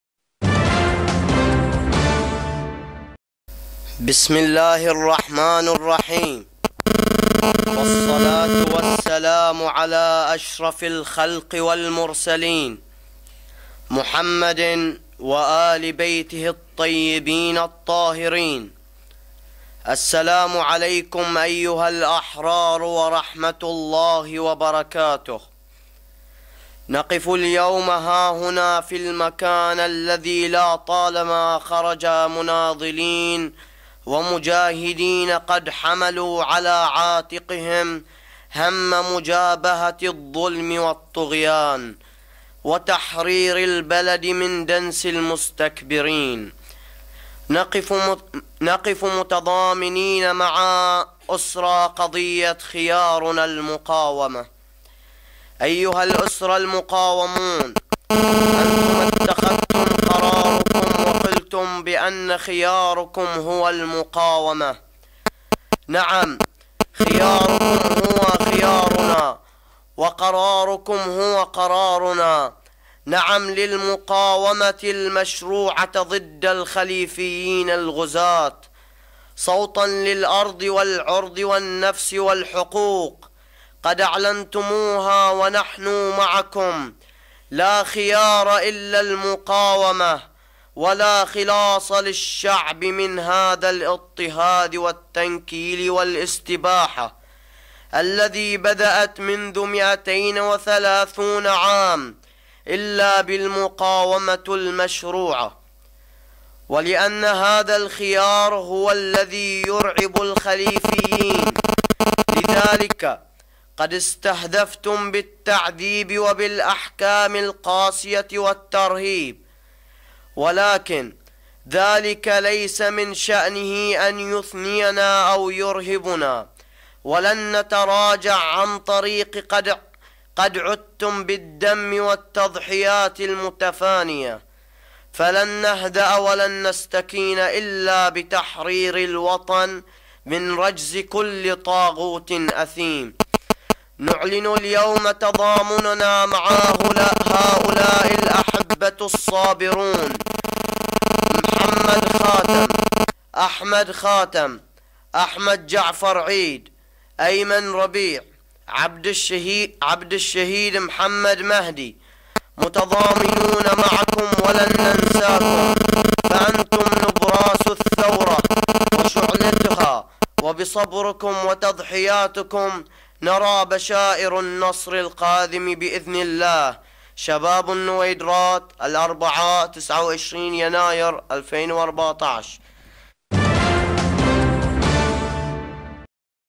كلمة شباب النويدرات في الوقفة التضامنية مع معتقلي قضية "خياررنا المقاومة" 29/1/2014 Bahrain
كلمة شباب النويدرات ضمن أسبوع الوفاء والتضامن مع معتقلين قضية "خيارنا المقاومة" حيث أقام شباب النويدرات جنوب العاصمة المنامة الوقفة التضامنية مع المعتقلين مساء أمس يوم الأربعاء (29 يناير/كانون الثاني 2014) , وشارك في الوقفة عدد من الأهالي مطالبين بالأفراج الفوري عن المعتقلين .